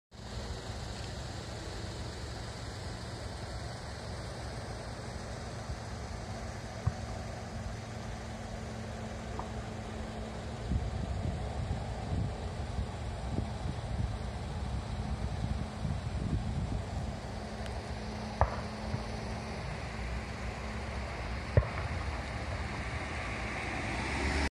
ウェザーリポート動画1121@長野県小布施町(16:05)上が広がった雲
志賀高原方面にかかる鉄床雲みたいに上が広がった雲